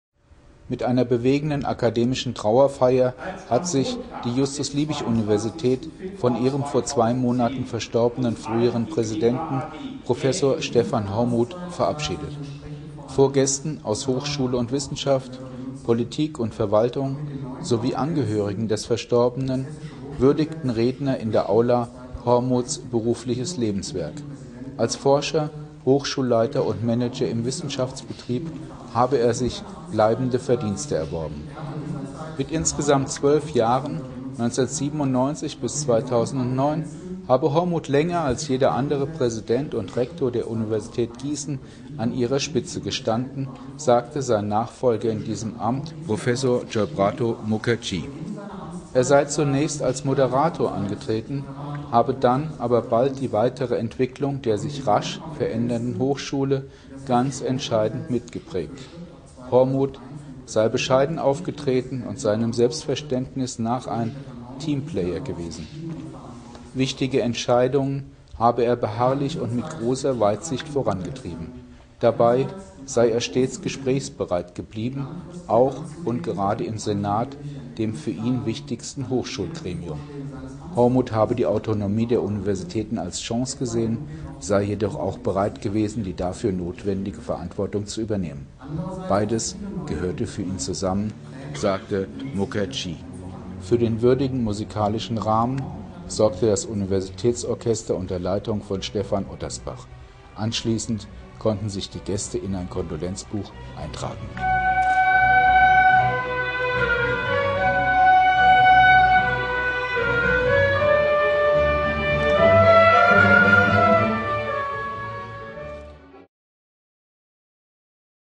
Trauerfeier für ehemaligen Uni-Präsidenten Prof. Stefan Hormuth
Mit einer bewegenden Akademischen Trauerfeier hat sich die Justus-Liebig-Universität von ihrem vor zwei Monaten verstorbenen früheren Präsidenten Prof. Stefan Hormuth verabschiedet.